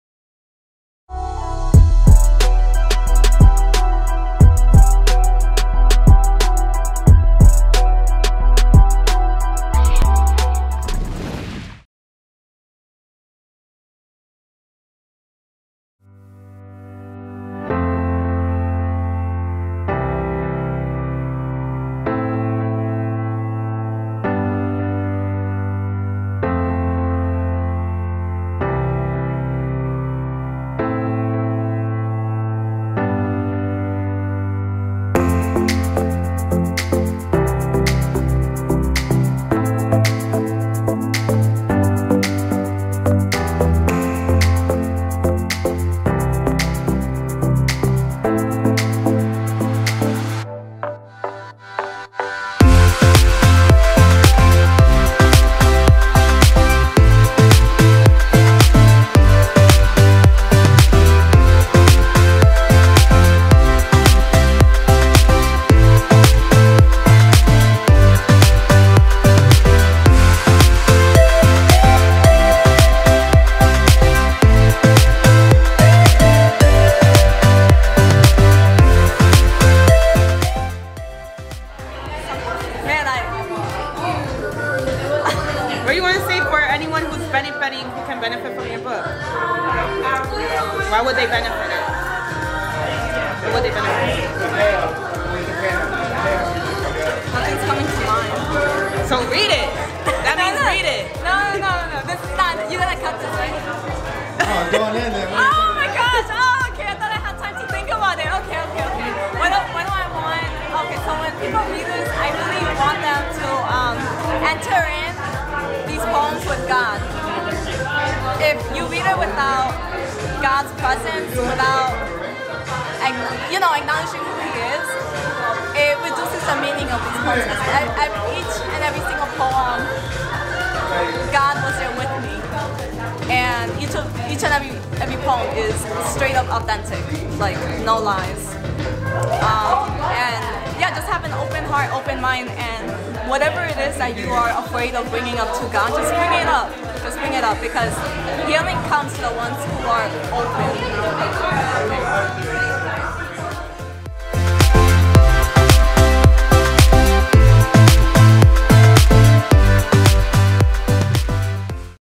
Poem Book Release Recap
A couple of weeks ago I had the opportunity to capture some great moments at the Poem Book Release of Songs of Freedom Authored by Alexandra Henry and Cover Artwork by Theresa Flaherty.
Both came together to work on this poem book and convey the message of God through their art.
This video is a quick recap of the "Meet The Author" of Songs of Freedom event hosted at St. Helena's Church in the Bronx. She gives her perspective on working on this project and invites you to also check out the Poem Book.

Get a copy of the Collection of Poems on Amazon on both Paperback and Kindle formats

Have a great Eastertide Season and May you grow closer to Christ.

Remember Stay Blessed, God Bless!!!